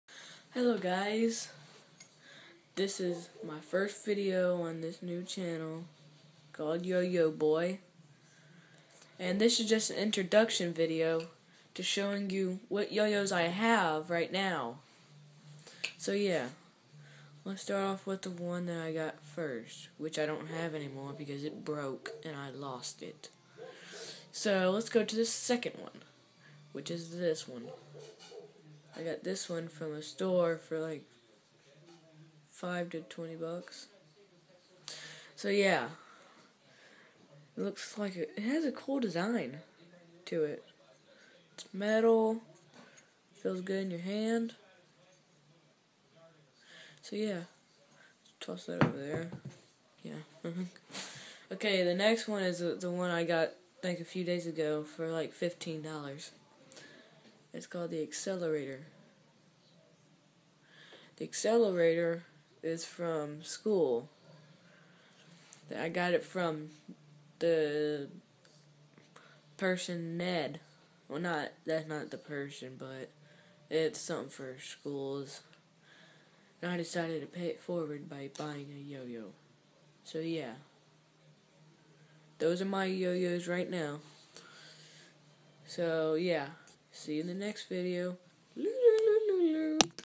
Introduction to my channel
This is my first video on my new channel so it's gonna be an introduction video